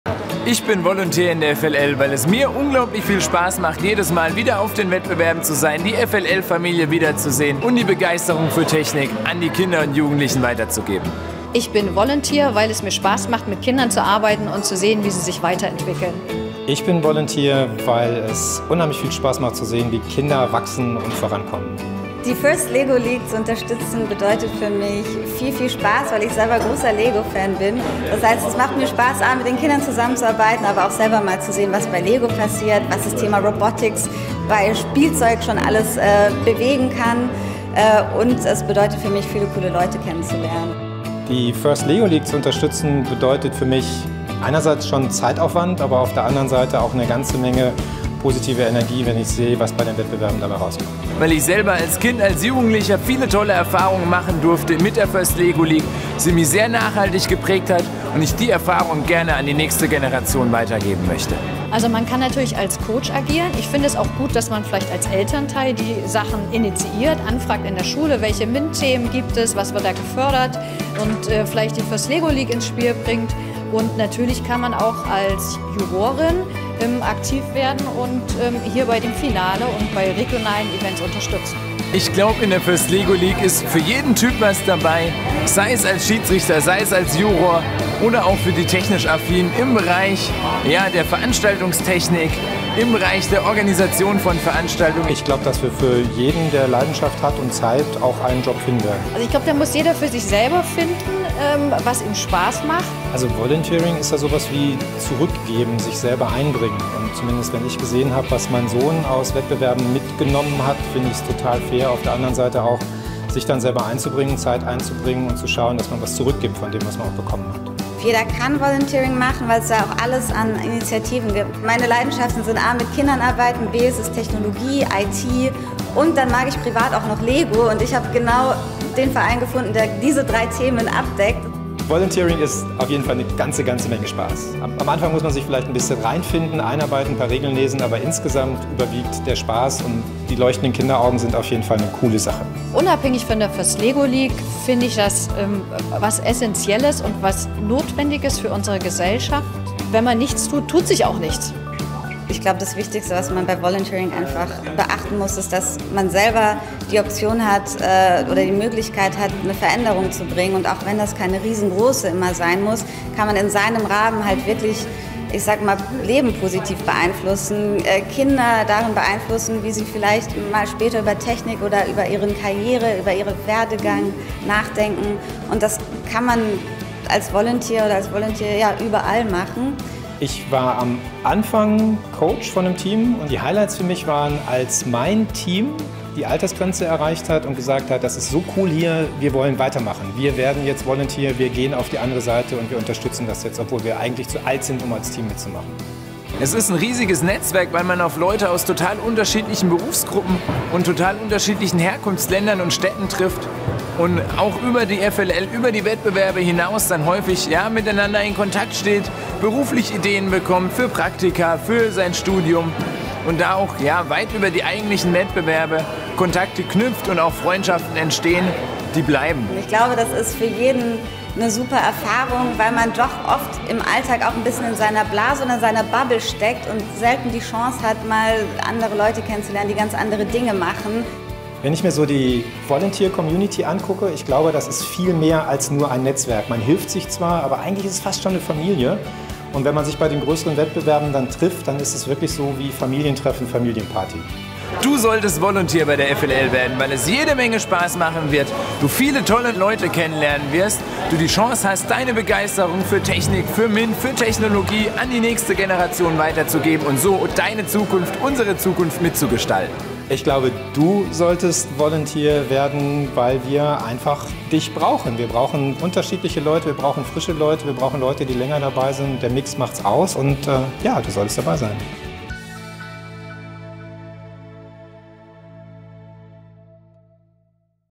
Mach mit als Volunteer bei FIRST LEGO League!
Lass dich von erfahrenen Volunteers inspirieren, zum Beispiel als Explore GutachterIn, Challenge Jurymitglied oder SchiedrichterIn ein FIRST LEGO League Event zu unterstützen!